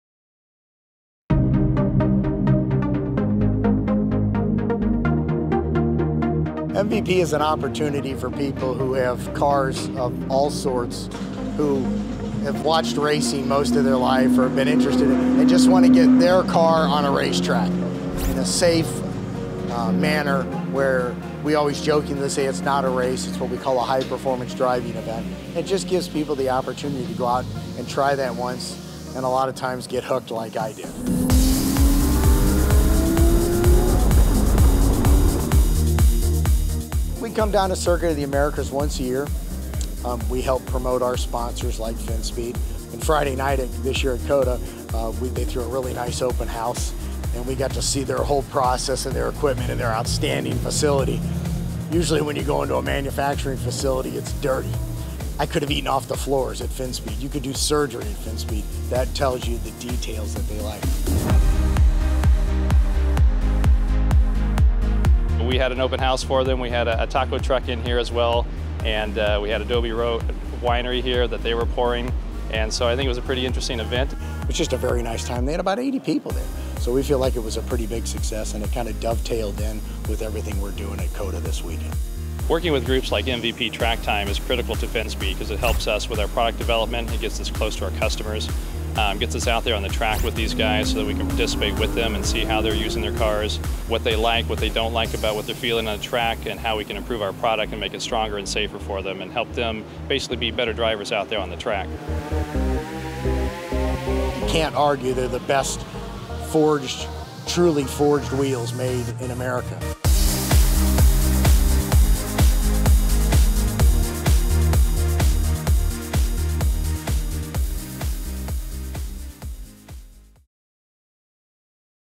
Finspeed MVP | COTA 2019 Open House
Finspeed is proud to partner with high caliber motorsport groups like MVP Track Time. As the official wheel partner of MVP, we recently hosted an open house for the entire MVP paddock at our facility in Austin, TX. We always like giving our partners and customers a sneak peek into what it takes to make a world class motorsport wheel.